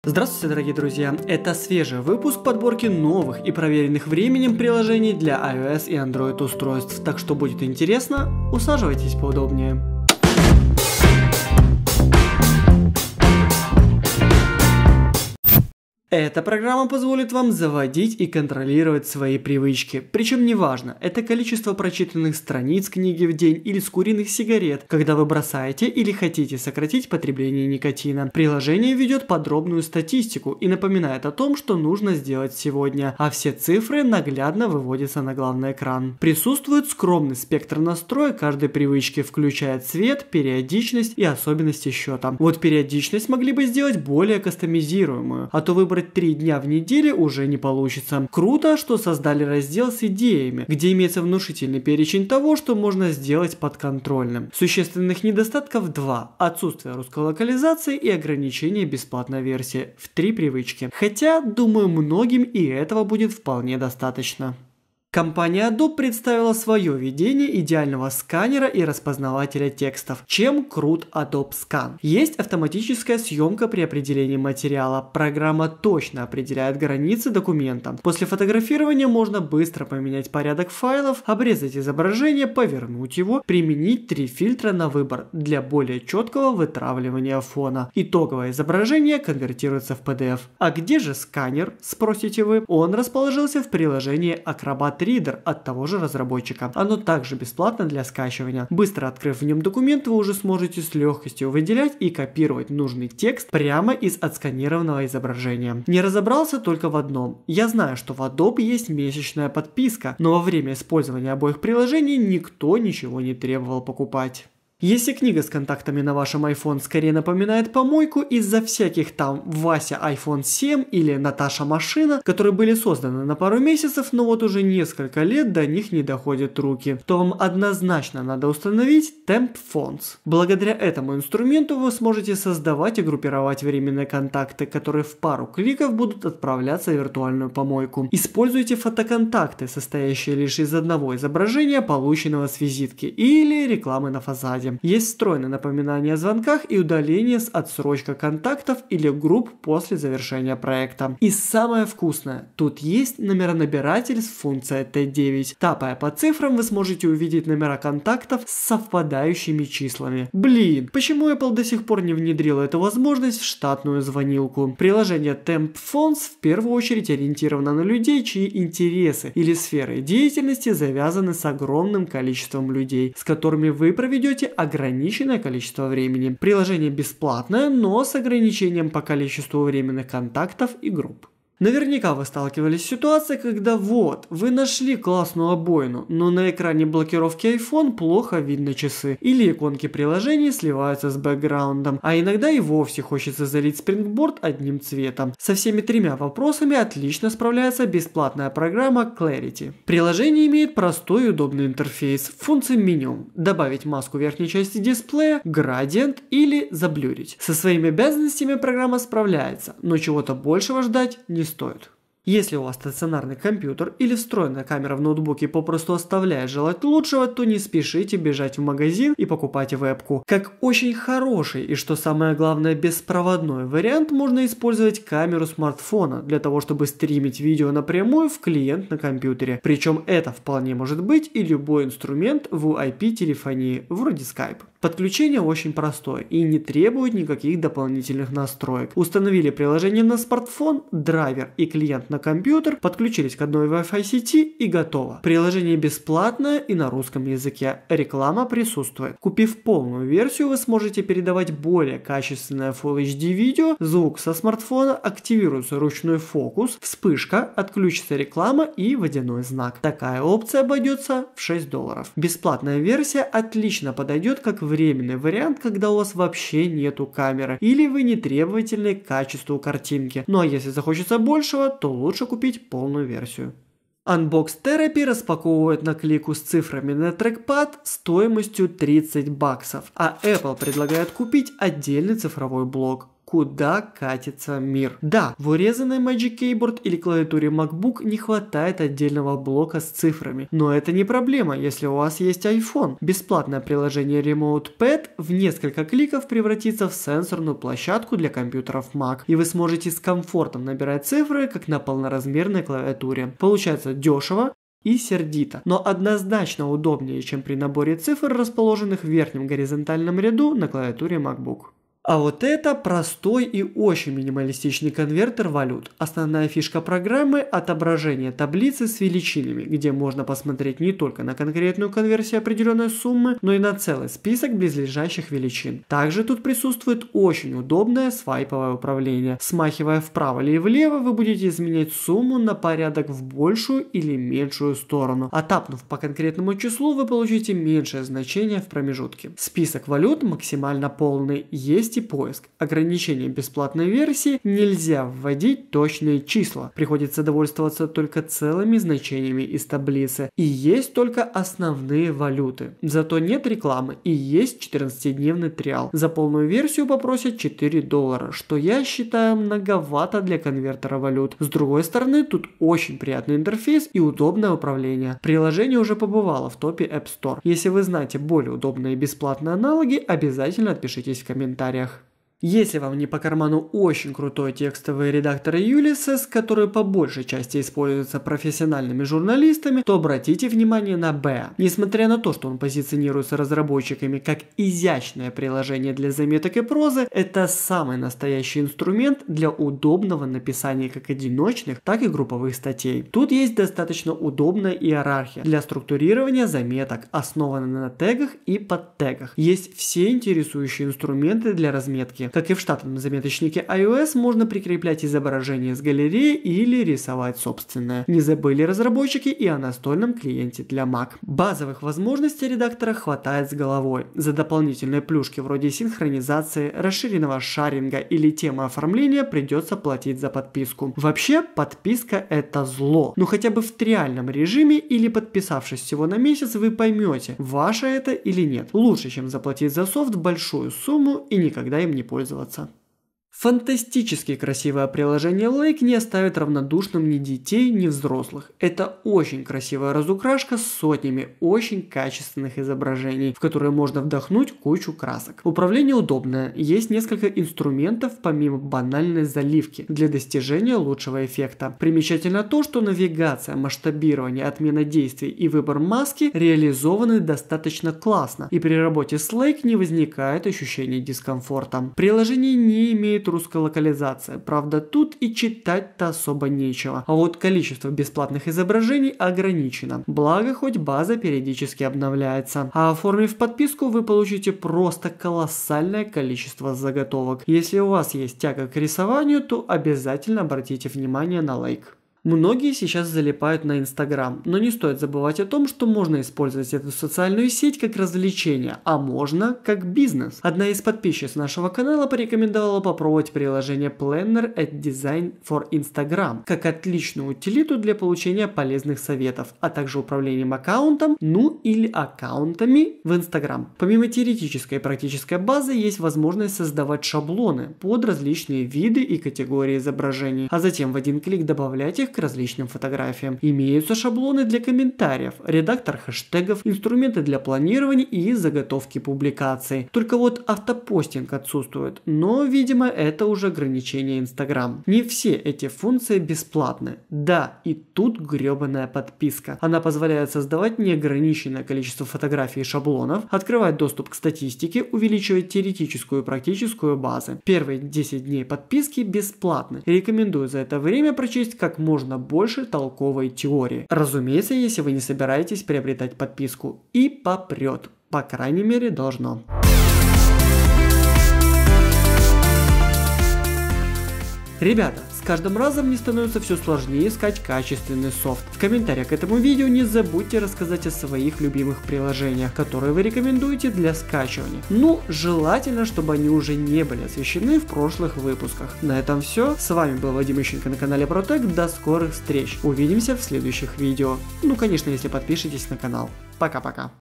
ТОП-10 качественных приложений для iOS и немного для Android |№8 ProApps от ProTech
Вы на канале ProTech и это ТОП-10 качественных приложений для iOS и немного для Android. Восьмой выпуск лучших приложений для iOS и Андроид устройств, тщательнейшим образом отобранных из App Store и Google Play. Большая часть программ бесплатная.

ВСЕ НАЗВАНИЯ ПРОГРАММ И ССЫЛКИ НА СКАЧИВАНИЕ В ПРИКРЕПЛЕННОМ КОММЕНТАРИИ!

Tally позволит контролировать привычки и постепенно избавиться от них. Adobe Scan отлично сканирует документы с применением фильтров, а если установить Adobe Acrobat, то вы сможете распознавать текст с фотографий. TempPhones сгруппирует временные контакты, а также позволит искать контакты на iPhone путем ввода номера на цифровом блоке. Clarity в несколько кликов затемнит верхнюю часть изображения, нанесет на него блюр для создания идеальных обоев для iPhone. Также есть инструмент для заливки спрингборда. EpocCam позволит сделать из iPhone веб-камеру для компьютера. Remote Keyboard Pad превратит iPhone в цифровой блок для Maс, которого так не хватает на клавиатуре. Elk - красивый и удобный конвертер валют. Bear - отличный заметочник с внушительной функциональностью и разметкой Markdown. C разукрашкой Lake скучать не придется. Planner & Design for Instagram поможет превратить Instagram-аккаунт в бизнес.